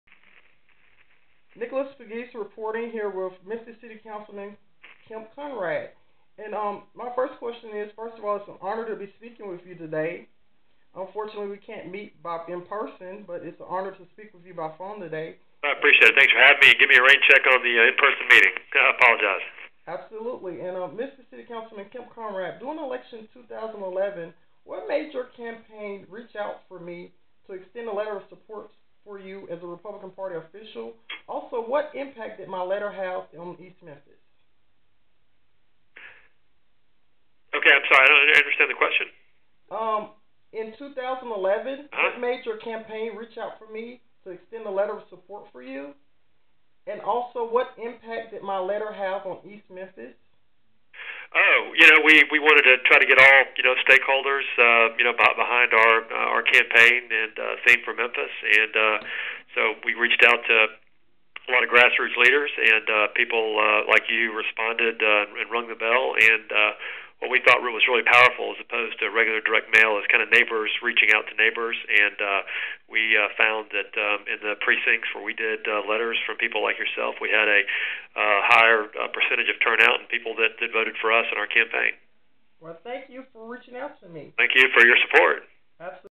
Phone Interview Part 1 With Memphis City Councilman Kemp Conrad
CNN iReporter Nicholas Pegues phone interview with Memphis City Councilman Kemp Conrad.Mr. Pegues is a former member of the Shelby County Republican Party Primary Board and is a current member of the Shelby County Republican Party Outreach Committee.In 2011 Mr. Pegues was the youngest Shelby County Unified School Board Applicant and was nominated to represent East Memphis/Germantown,Tennessee.